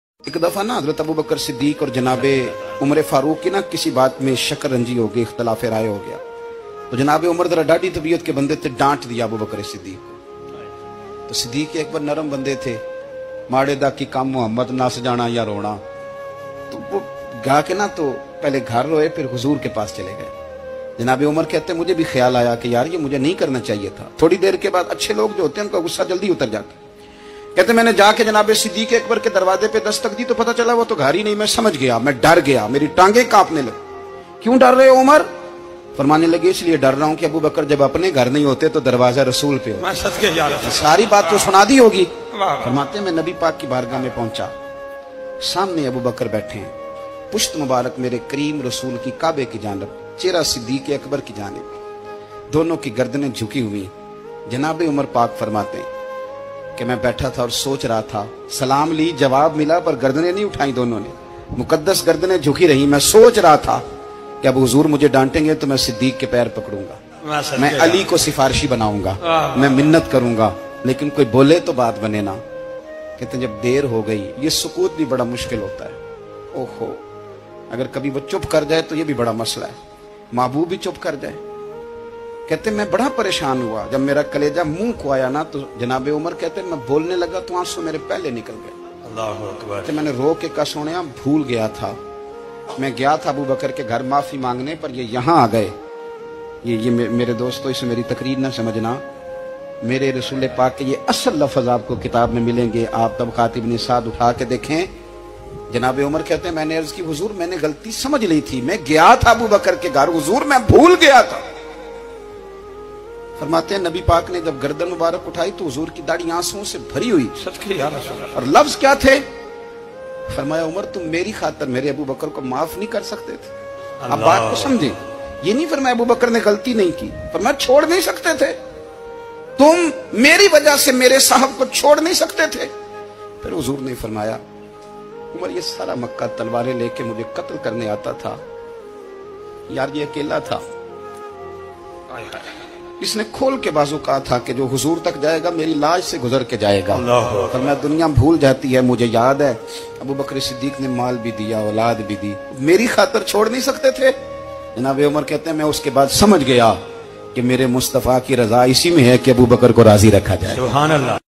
ایک دفعہ عمر فاروق اور ابو بکر صدیق کے درمیان اختلاف رائے ہو گیا😱 | Peer Ajmal Raza Qadri❣️| Islamic
Islamic Video
Peer Ajmal Raza Qadri ❣️
Beautiful Bayan
Hazrat Umer Farooq
Hazrat Abu Bakkar Sadiq